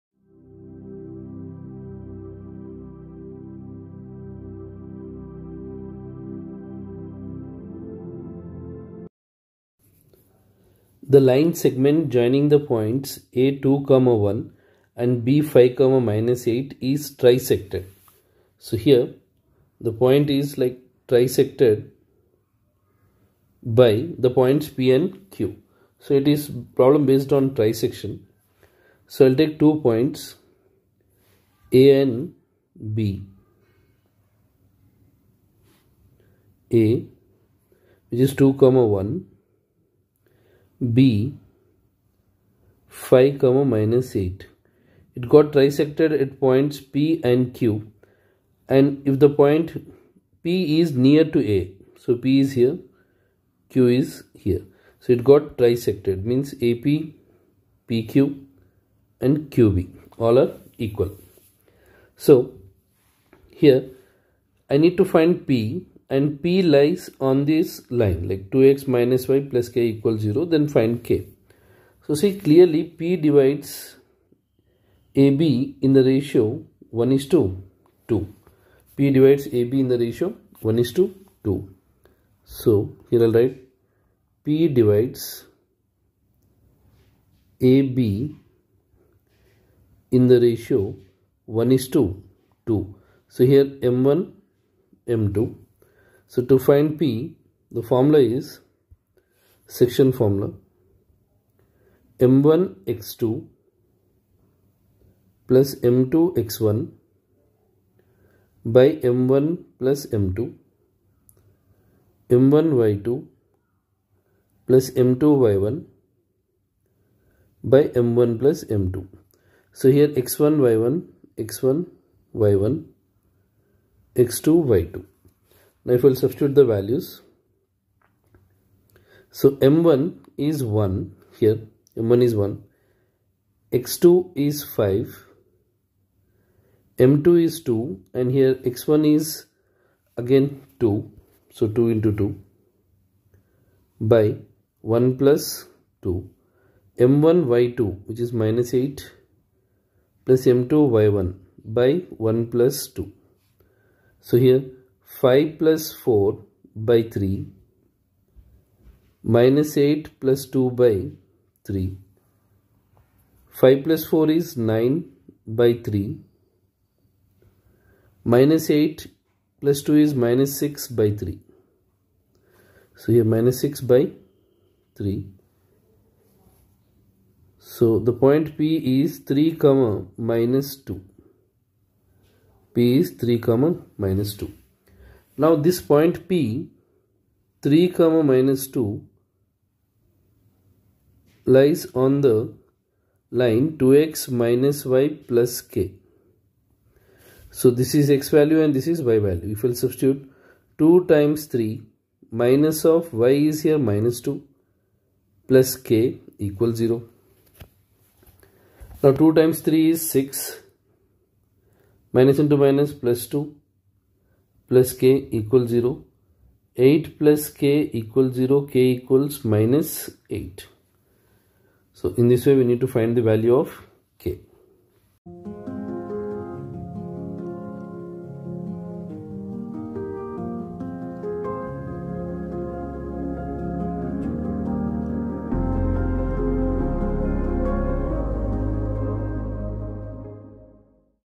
The line segment joining the points A (2, 1) and B (5, -8) is trisected by the points P and Q
The line segment joining the points A (2, 1) and B (5, -8) is trisected by the points P and Q, where P is nearer to A. If the point P also lies on the line 2x – y + k = 0, find the value of k.